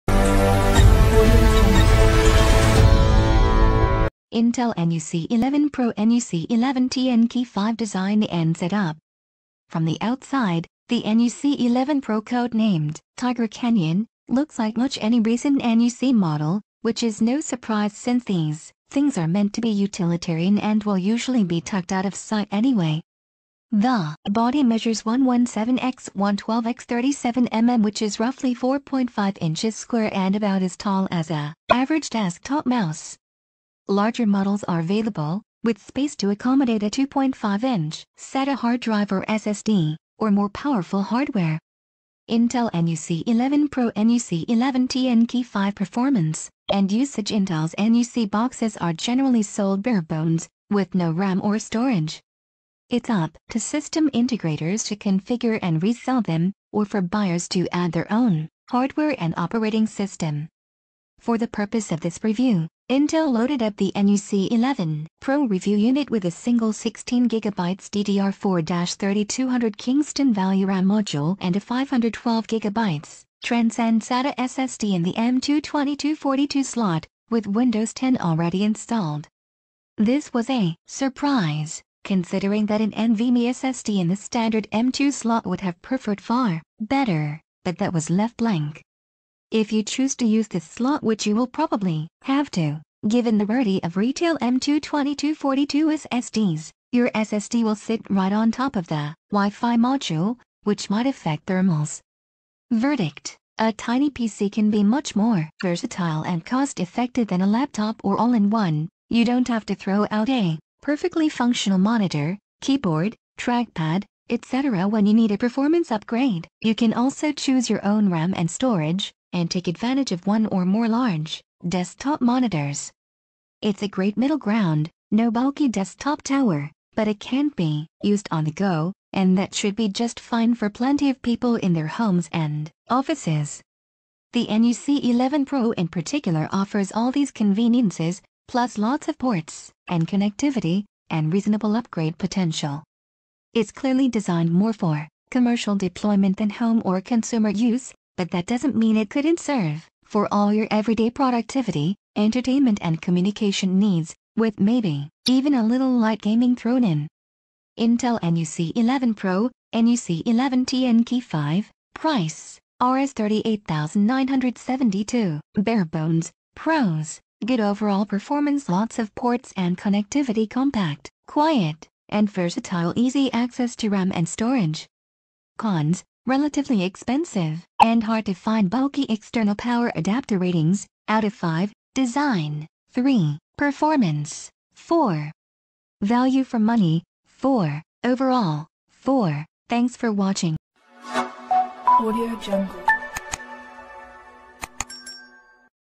Intel NUC 11 Pro (NUC11TNKi5) Review: Tiny Package, Big Potential
Do today's tiny PCs have enough power to get all your work done?

HIGHLIGHTS

This palm-sized PC has an 11th Gen 'Tiger Lake' Core i5 laptop CPU

There are plenty of ports and even some internal slots for expansion

While hard to find, the NUC might be ideal for certain niche use cases

Intel NUC 11 Pro (NUC11TNKi5)
Price: Rs. 38,972 (barebones)

Pros

Good overall performance

Lots of ports and connectivity

Compact, quiet, and versatile

Easy access to RAM and storage

Cons

Relatively expensive and hard to find

Bulky external power adapter

Ratings (out of 5)
Design: 3
Performance: 4
Value for Money: 4
Overall: 4